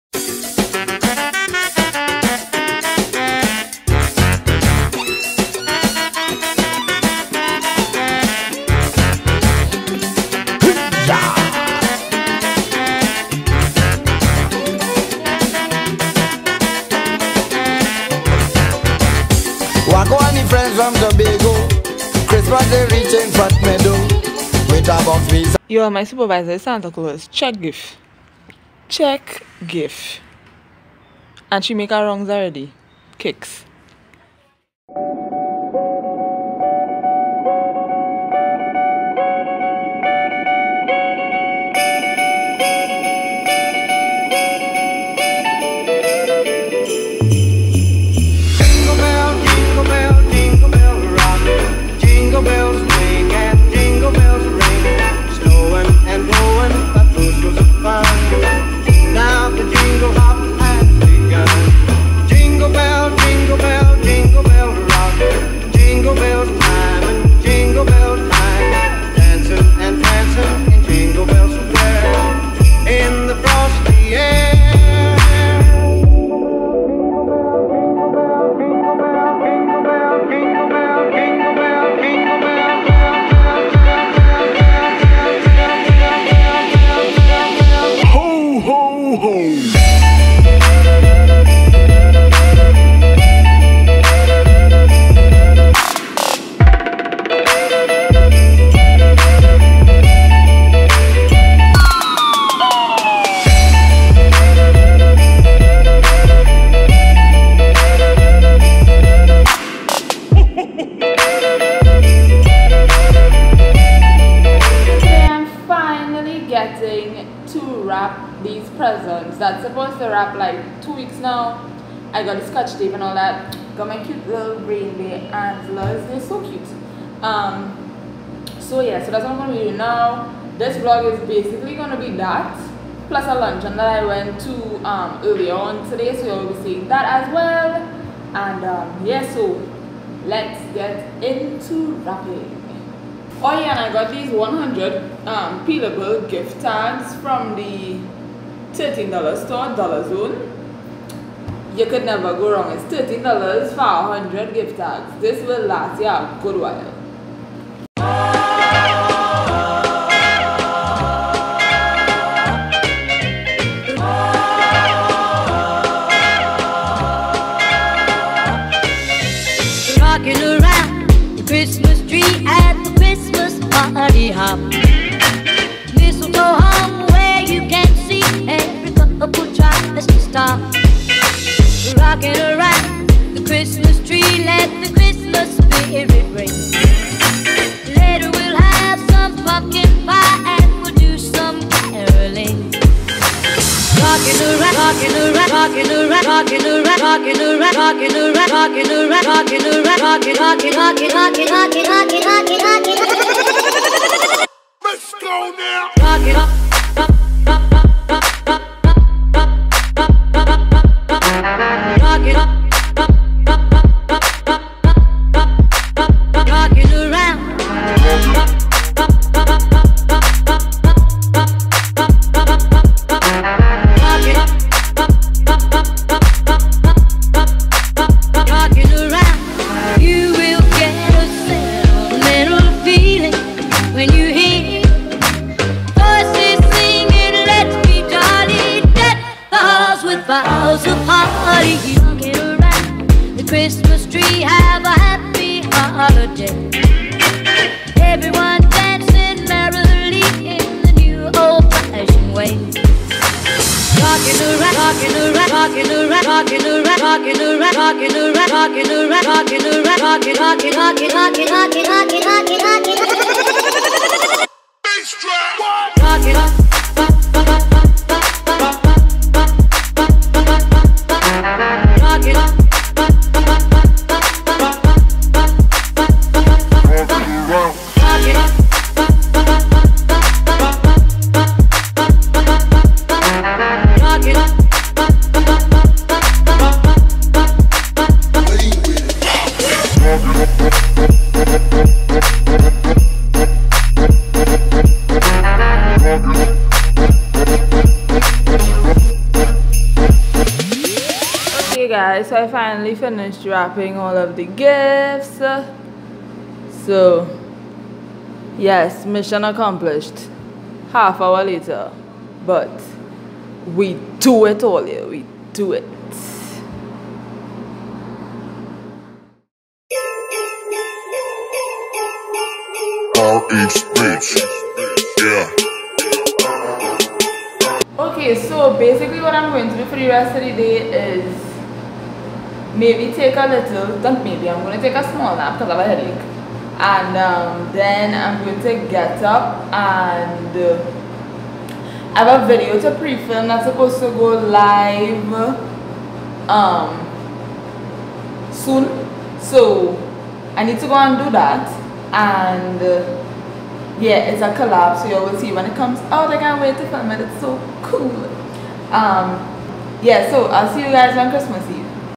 VLOGMAS Day 23 -Wrapping Presents Finally!!
HEY KINKLETS!

Let's wrap some gifts.

Videography: Me
Camera Used: Samsung HMX F-90

Thanks for taking the time out to Watch my videos... Much appreciated

XO!
Gabby